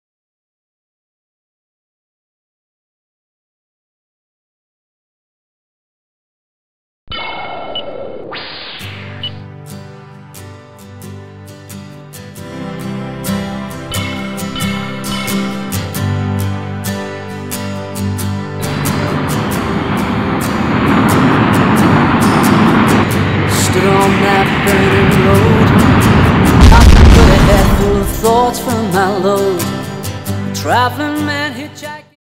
VFX Tests - Apple iTable & Iron Man Flight
Here's two new vfx tests, one is a concept for an Apple iTable, the other is a little Iron Man inspired flight.

Song is by Two Way Street.